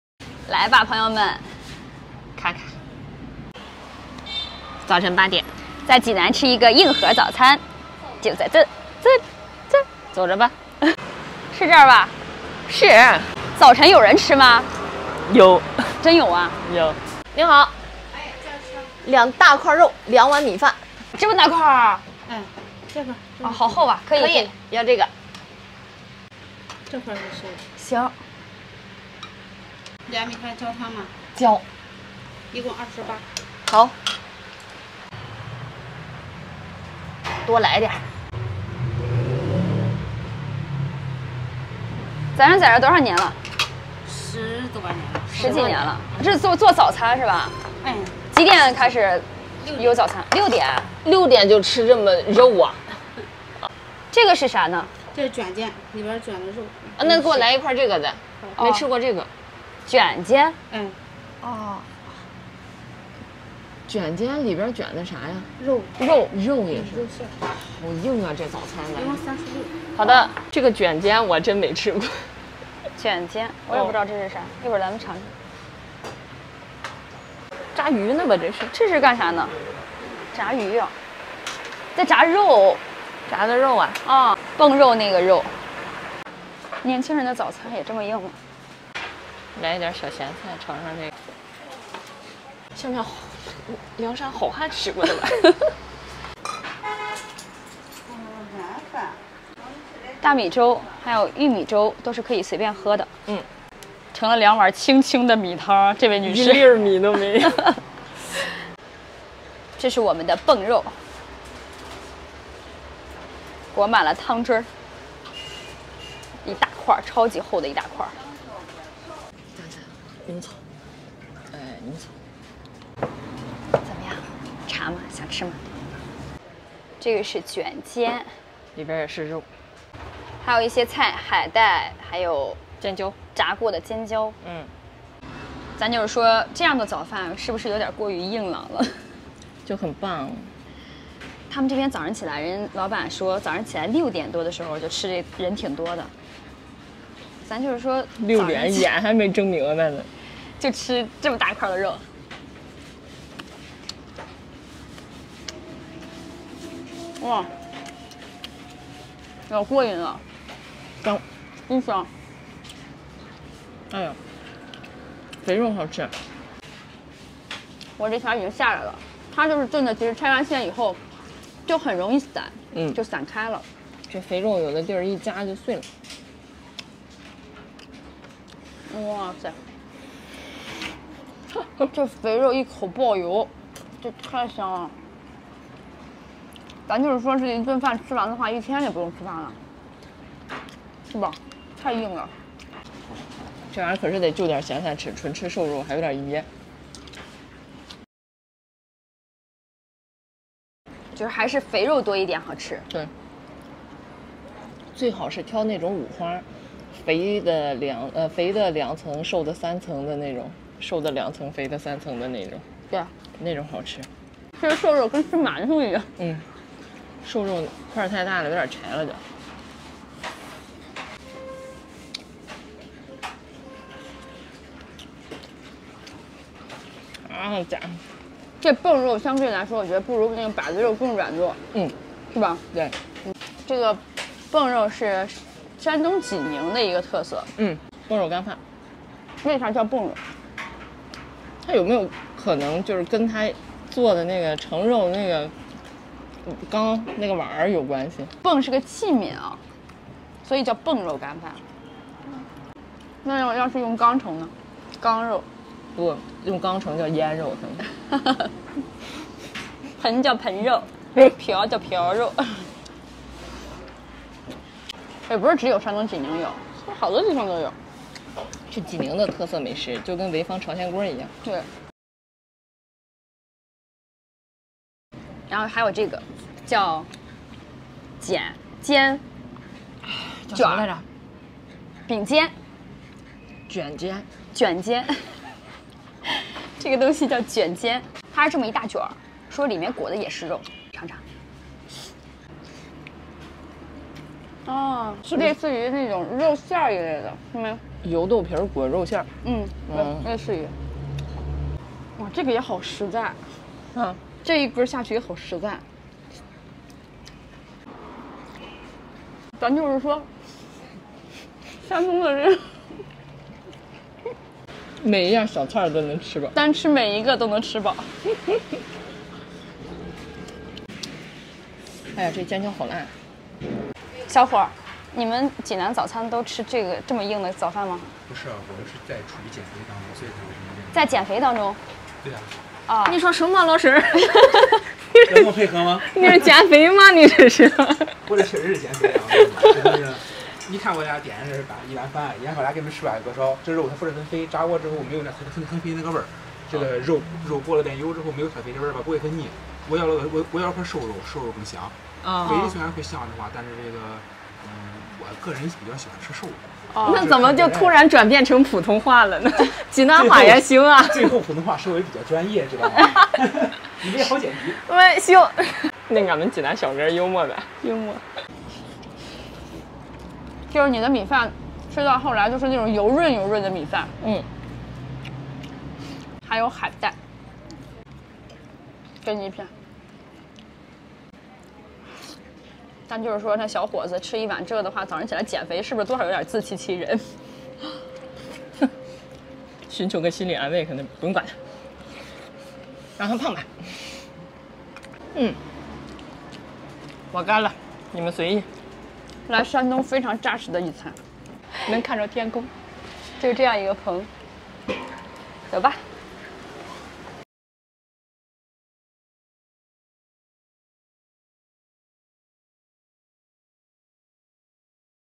【寻尝姐妹】山东济南硬核早餐，12元一块大肉配干饭，吃完一天不觉得饿【4K】
一个怎么吃都不胖的二姐，一个喝水都长肉的二妹，带你吃遍大江南北